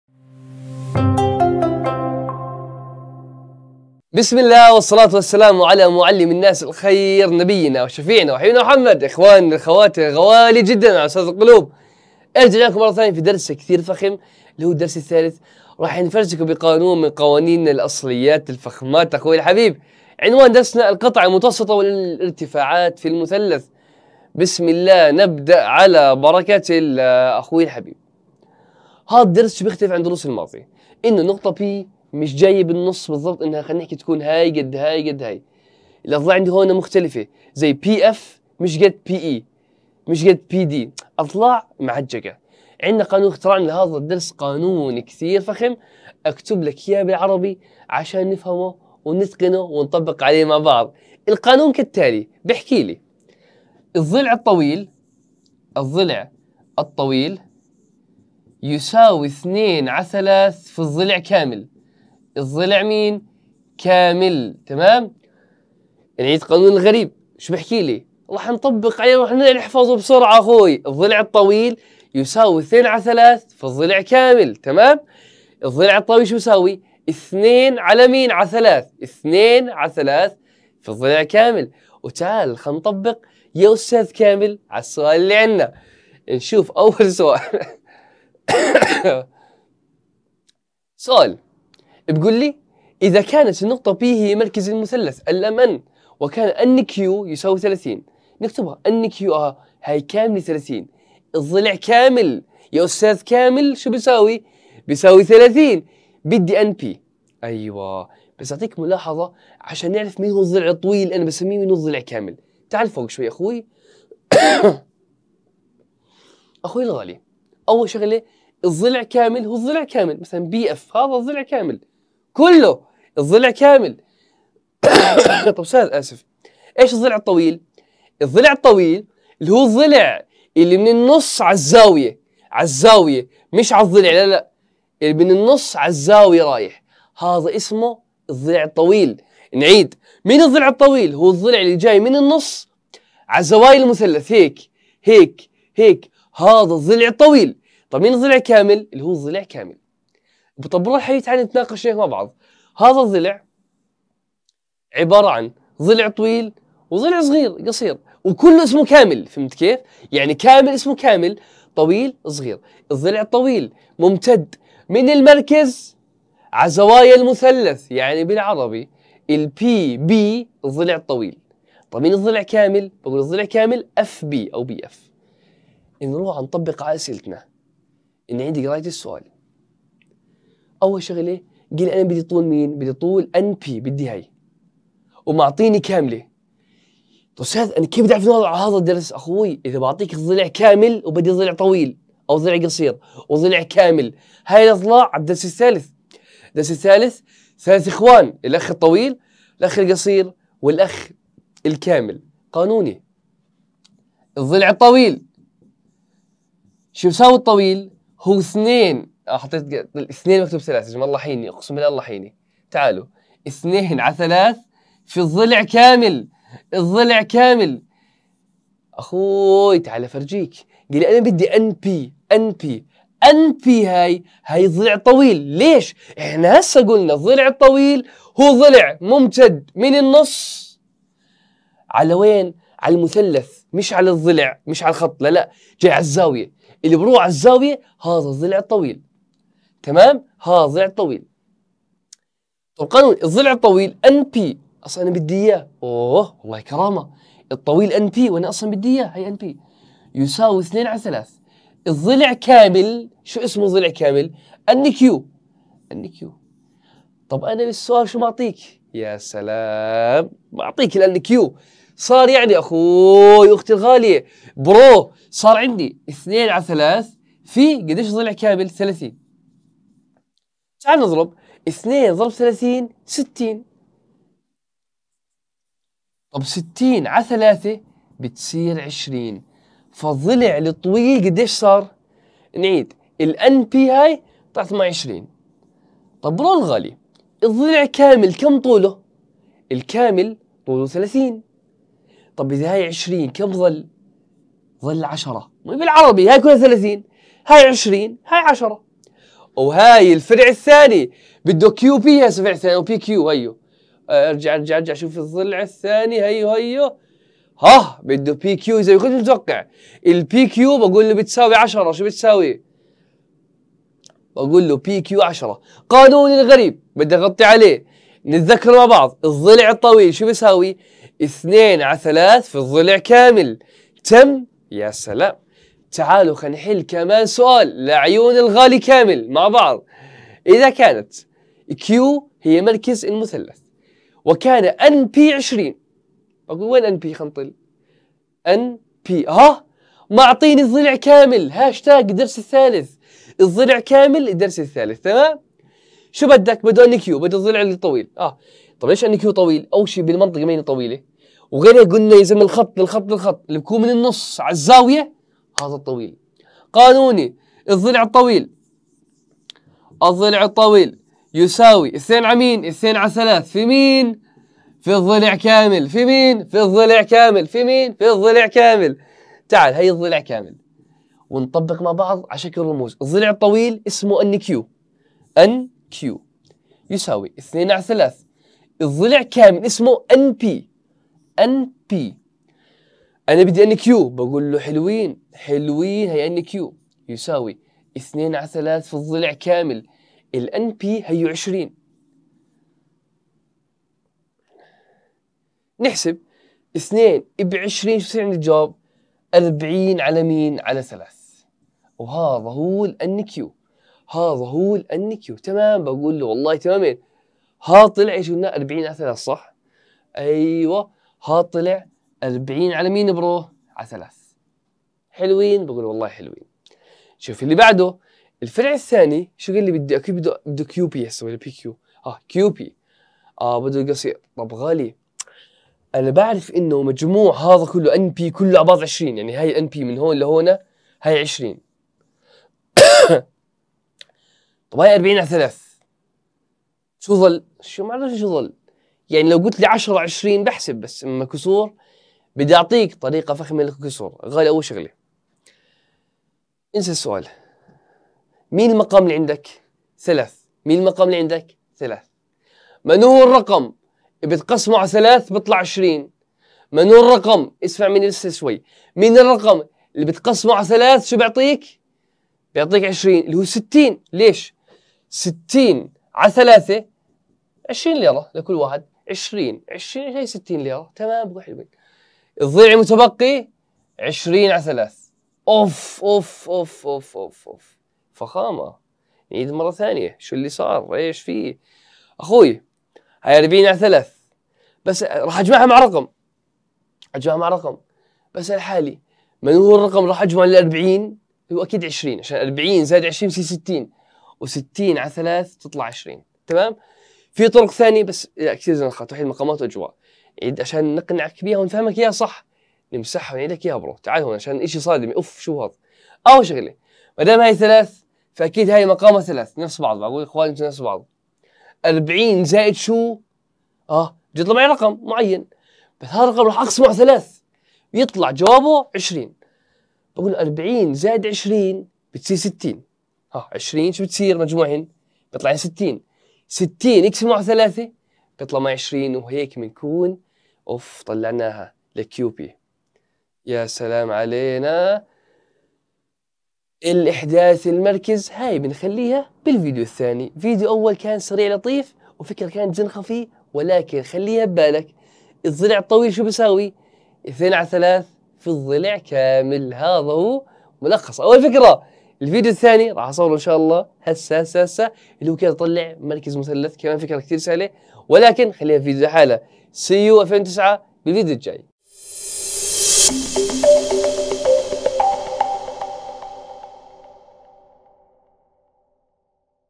القطع المتوسطة والارتفاعات في المثلث ج1 || الرياضيات || الصف التاسع || أ. راشد الفاعوري
- هاتف☎️: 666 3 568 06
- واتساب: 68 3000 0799